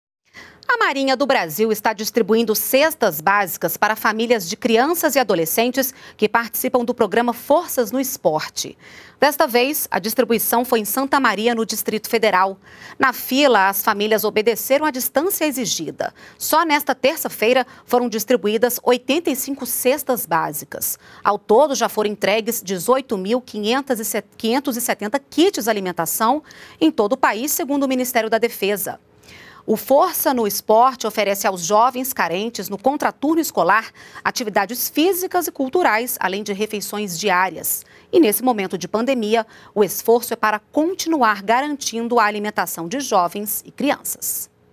Moradores do DF recebem cestas básicas da Marinha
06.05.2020 - A Marinha do Brasil está distribuindo cestas básicas para famílias de crianças e adolescentes que participam do programa Forças no esporte. Desta vez a distribuição foi em Santa Maria, no Distrito Federal.